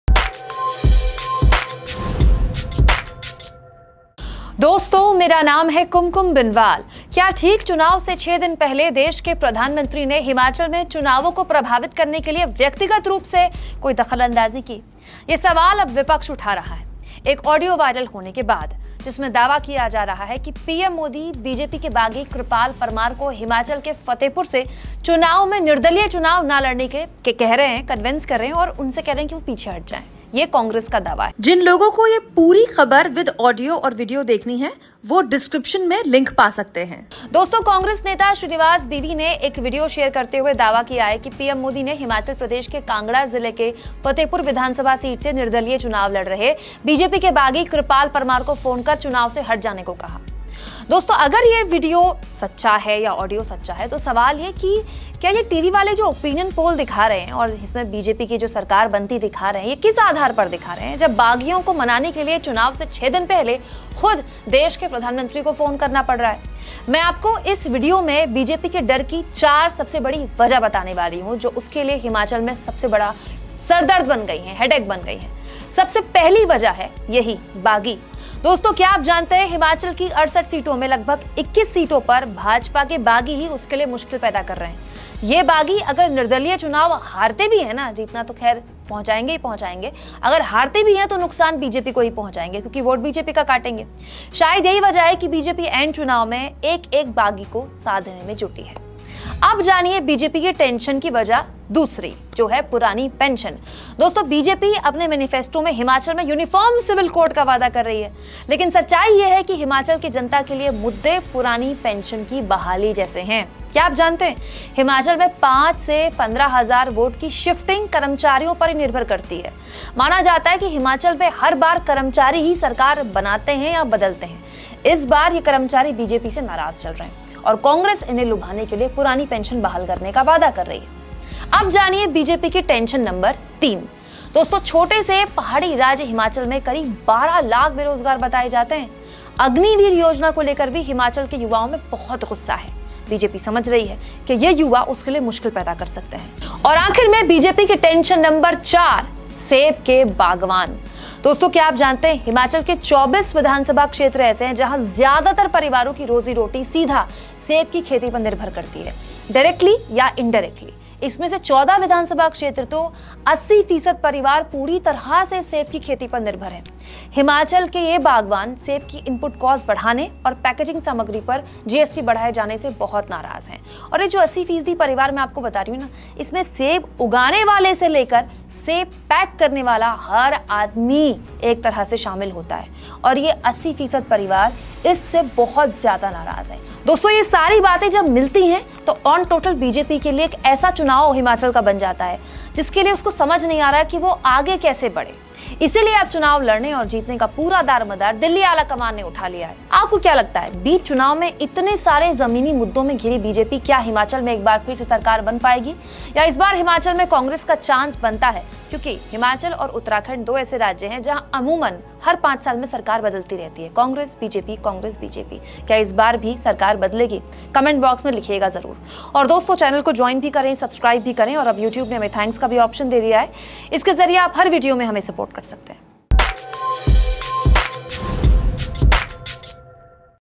PM Modi ने बागी को क्यों किया फोन? BJP की Inside Story | Modi Audio Viral, Kumkum Binwal Analysis
All the TV channels are showing the formation of the BJP government in Himachal Pradesh in their opinion polls, while due to issues like the anti-incumbency factor, inflation, and unemployment, the BJP may suffer a lot in the Himachal Pradesh elections. Kumkum Binwal's analysis of the Himachal Pradesh elections.